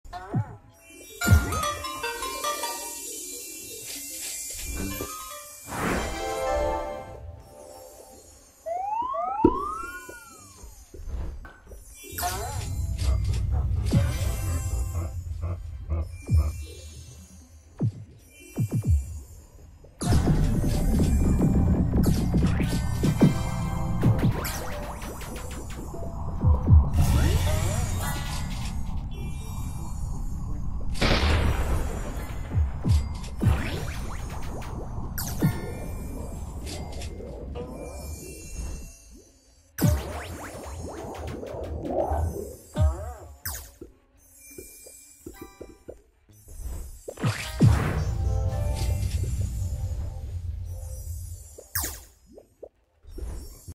Onett's robot party?
This showed up randomly when i was macroing pepper fields. When i stopped macroing to check if onett's in the server, he wasn't there.